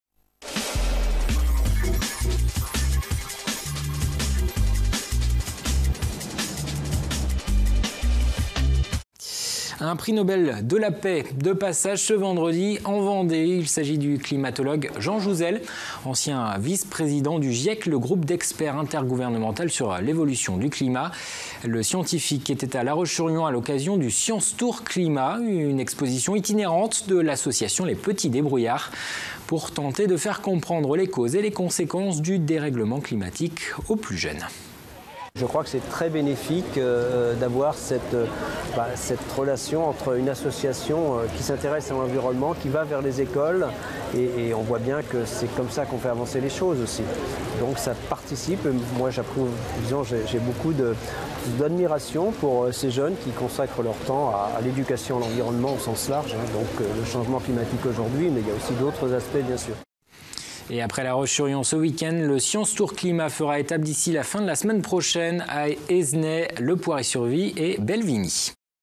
Jean Jouzel lance le Science Tour Climat 2016 à La Roche-sur-Yon
Reportage de TV Vendée, le 10 juin 2016, à l’occasion du passage de Jean Jouzel, Climatologue, pour le lancement du Science Tour Climat 2016. Source